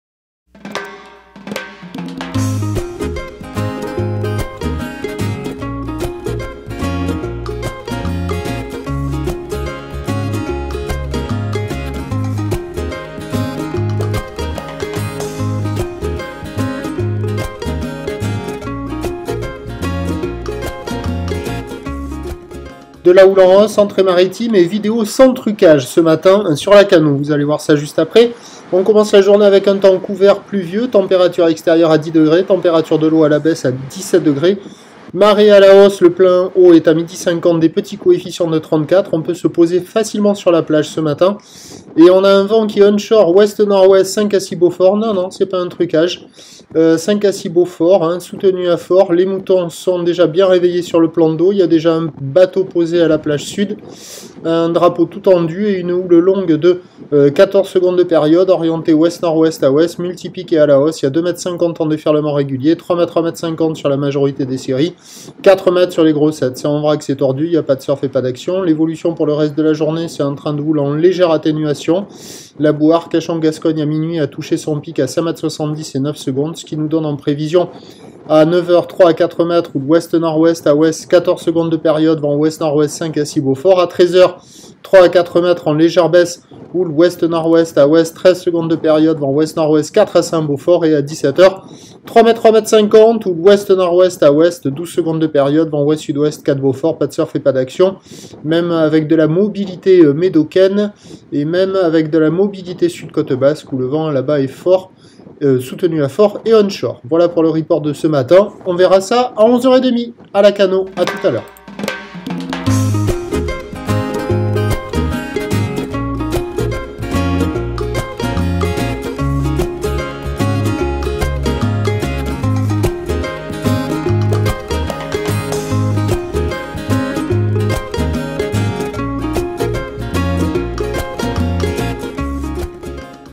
Lacanau Surf Report Vidéo - Mercredi 06 Novembre 8H10
Houle longue WNW-W en hausse, vent onshore soutenu à fort.